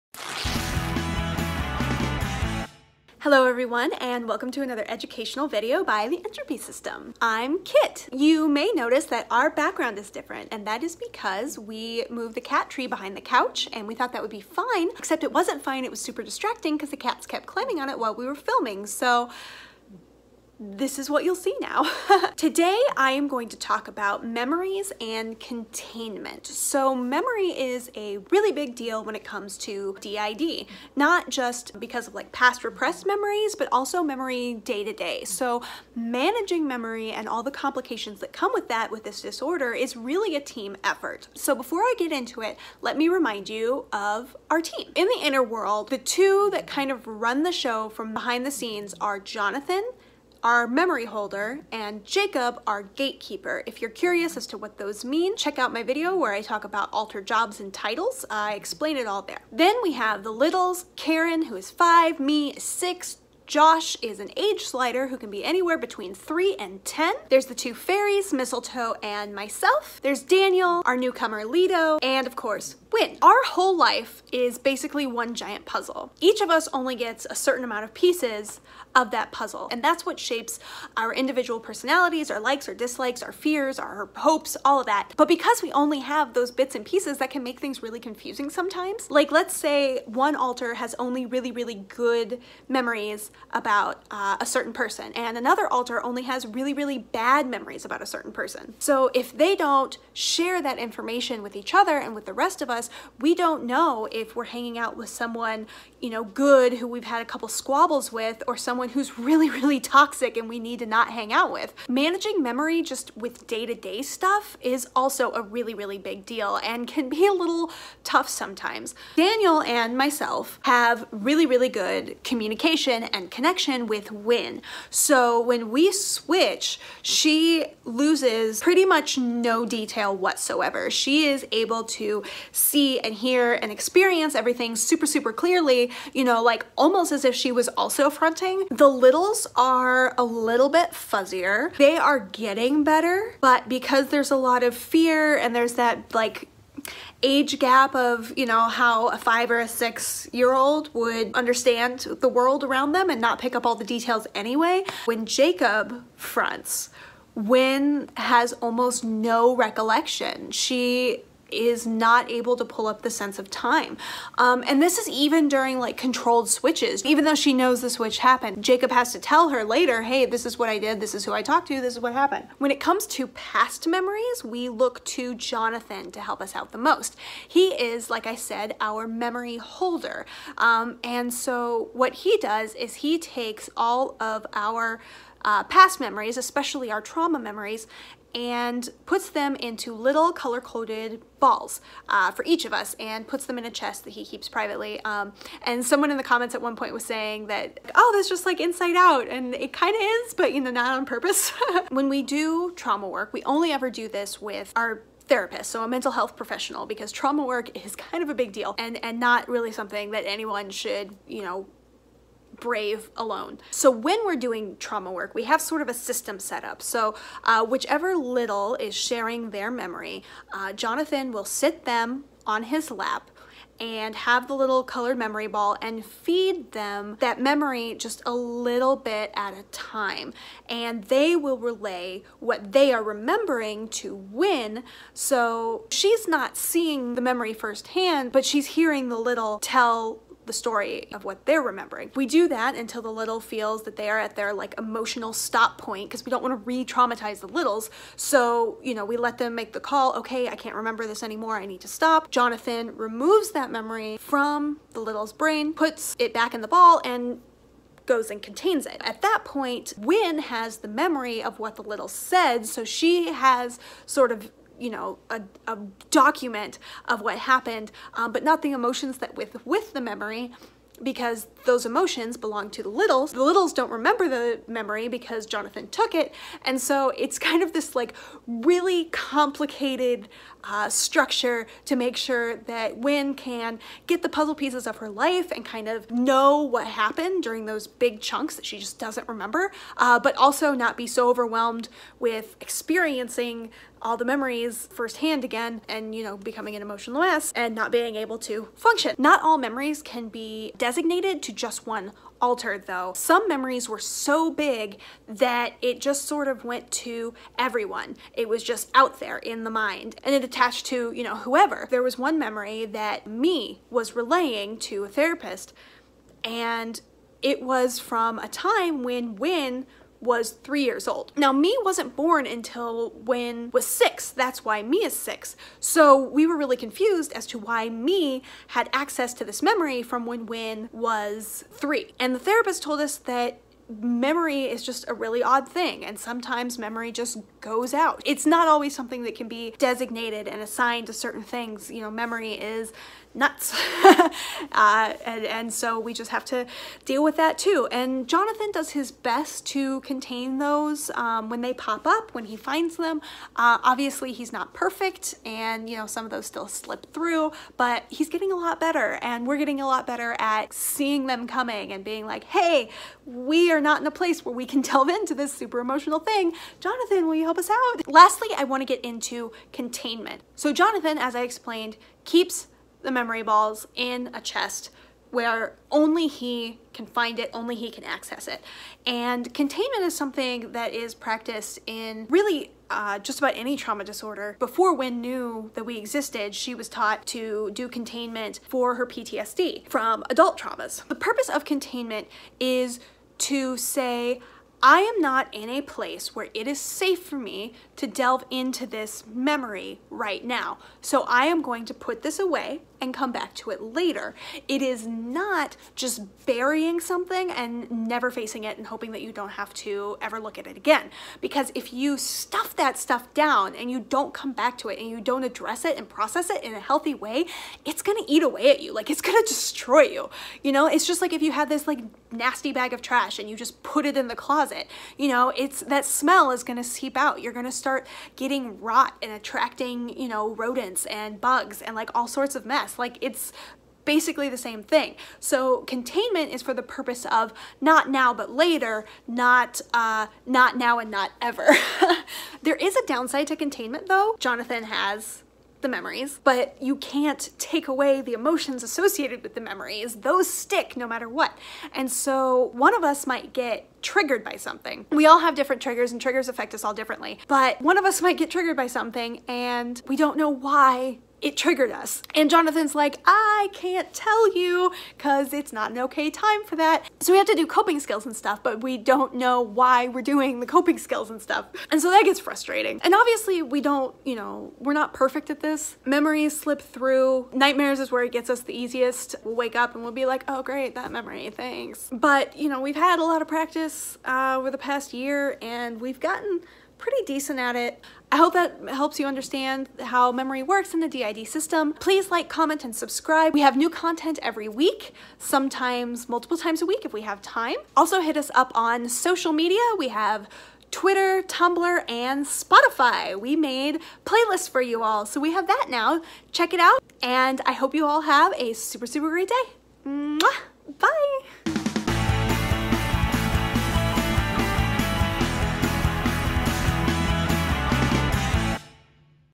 Memories and Containment
Kit explains how memory retrieval and containment works. And we sort of added animation! (It's our first go at it. It took days.)